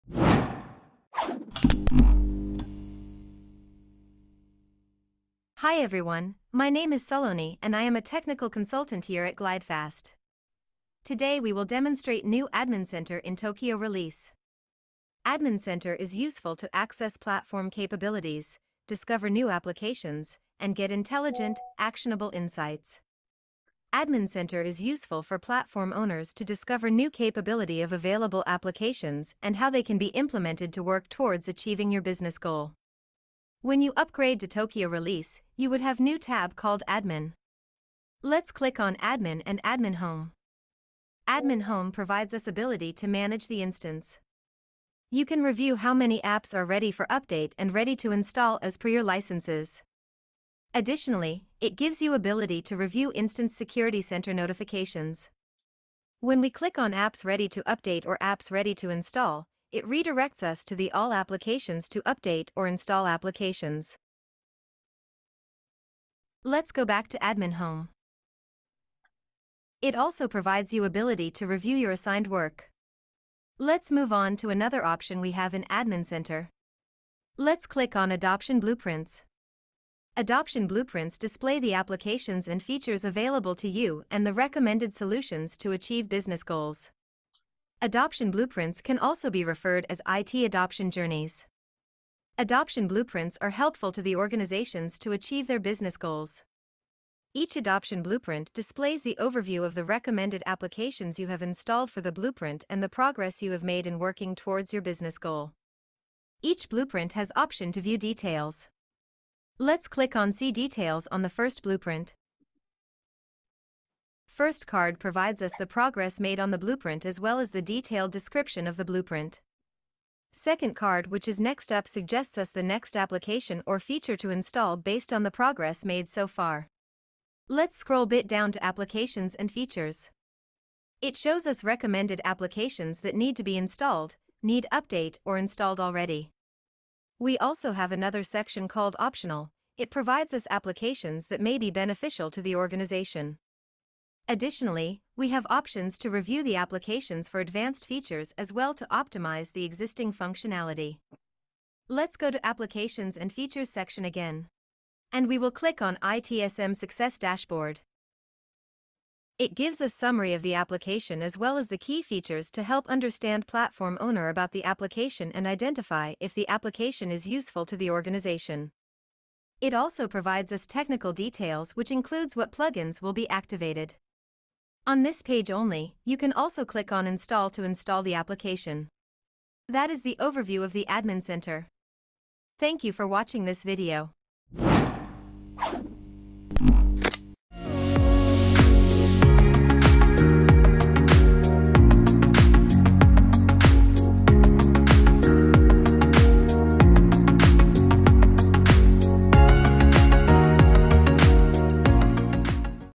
ITSM Tokyo Release Demo - Admin Center | Now Platform® Tokyo Release
Check out some of the latest ITSM features available in the Now Platform® Tokyo release! Technical Consultant Saloni Suthar will demo the Admin Center feature available with IT Service Management in the ServiceNow Tokyo release.

The ServiceNow® Admin Center application provides a central hub for platform owners and admins to easily access platform capabilities, discover new applications, and get intelligent, actionable insights. Admin Center is a new application in the Tokyo release.